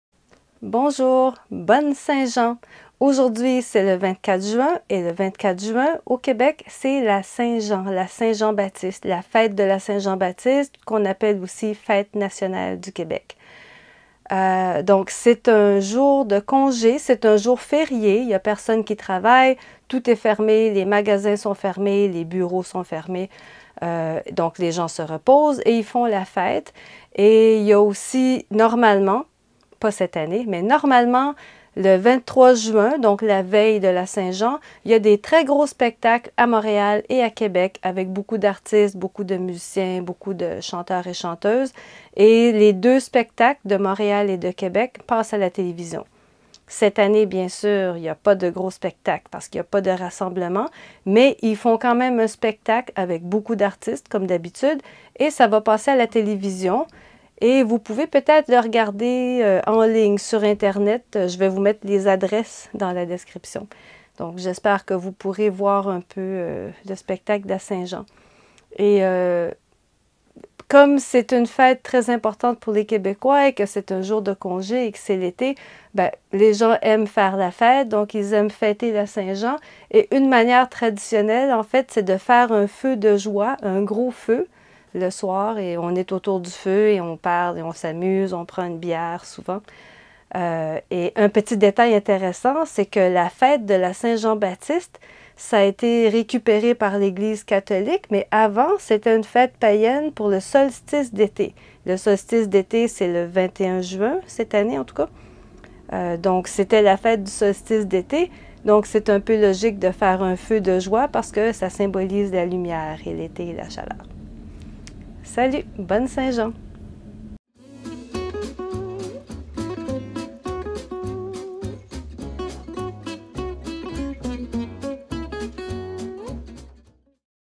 FRENCHPRESSO (Slow, Easy French) - Bonne Saint-Jean !
Le 24 juin, c'est la Saint-Jean-Baptiste, une des fêtes les plus importantes de l'année au Québec! On parle de son origine sur le site officiel de la Fête nationale du Québec.

Voici deux petites notes de prononciation:

- En français familier, on coupe souvent la fin des mots. Écoutez les mots "spectacles" et "artistes" à 0:35. Il y a deux consonnes finales, mais je prononce seulement la première: spectac', artis'.

- Écoutez comment je prononce "Montréal" à 0:36 et à 0:43. Vous savez probablement qu'on ne prononce pas le T, en français... Mais saviez-vous qu'au Québec, on prononce souvent "Moréal"? On peut même prononcer "Morial" - ou "Mourial", à la blague! Bien sûr, c'est la prononciation familière, pas standard.

Vous pouvez voir le spectacle de la Saint-Jean ici, si vous êtes au Canada ou que vous utilisez un VPN :